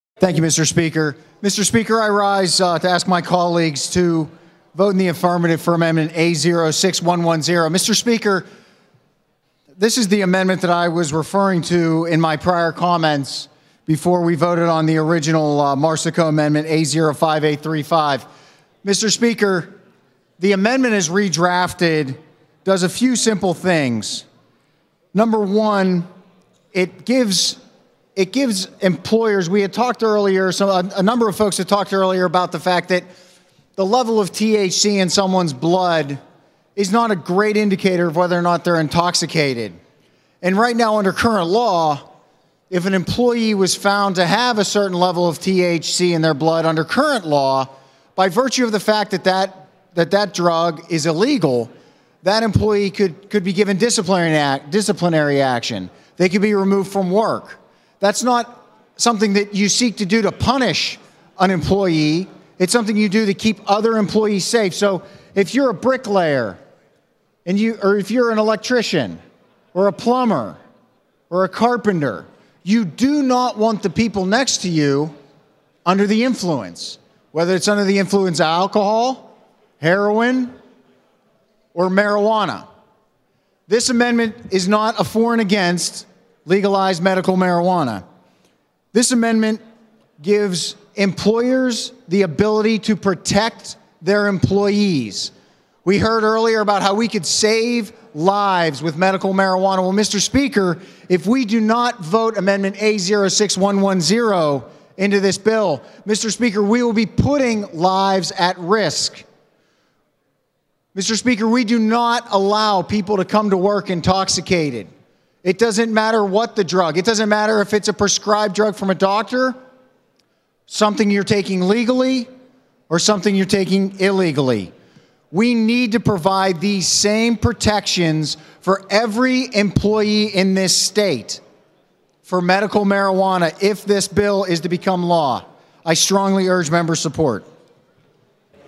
The Medical Marijuana Debate
Pa. State Rep. Eli Evankovich urges his House colleagues to support Amendment A06110 - which gives employers flexibility towards protecting employees if medical marijuana becomes legal.